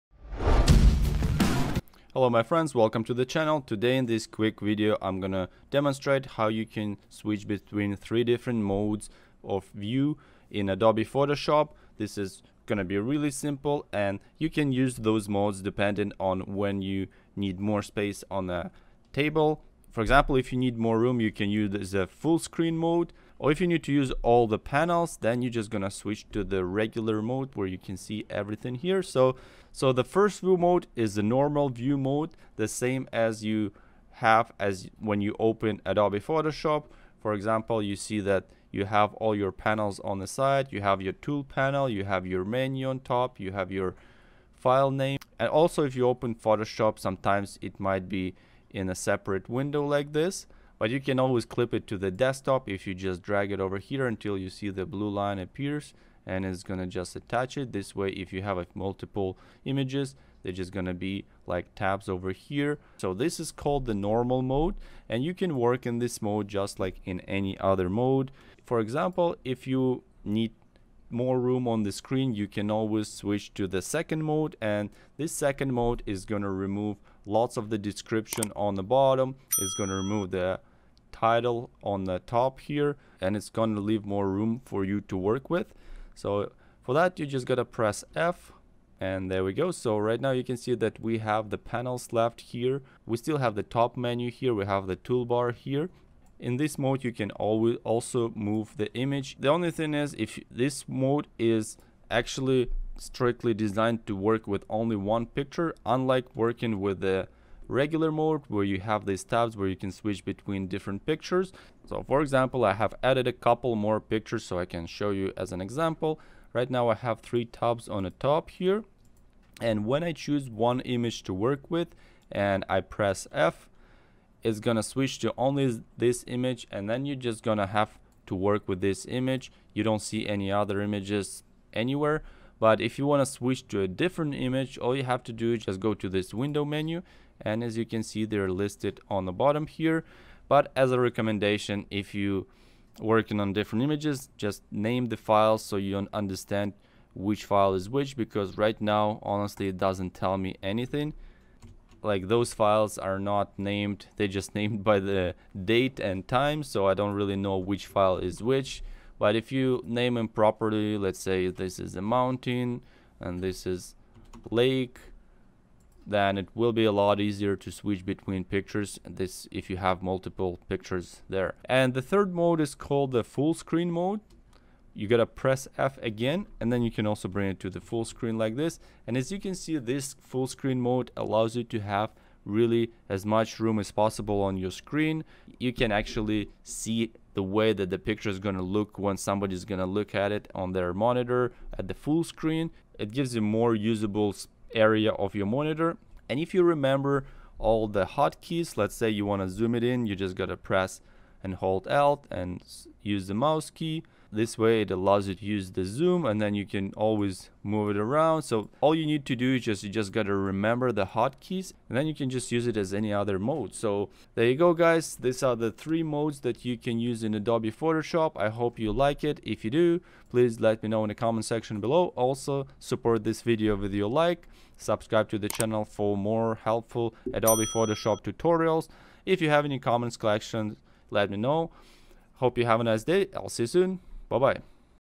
HOW TO SWITCH VIEW MODE IN ADOBE PHOTOSHOP
Did you know that you can choose from 3 different view modes in Adobe Photoshop? Well, in this video, I will show you how you can quickly switch between various viewing modes in Photoshop.
These modes are designed to help you utilize as much of the desktop space for the picture as possible. For example, if you don't need additional information about the file such as size, file name, etc, you can switch to another view mode that doesn't display all this info which gives you more room on the screen for the picture. And if you already know all your shortcuts and want to see how the picture will look at the full screen, then you can switch to a Full-Screen Mode.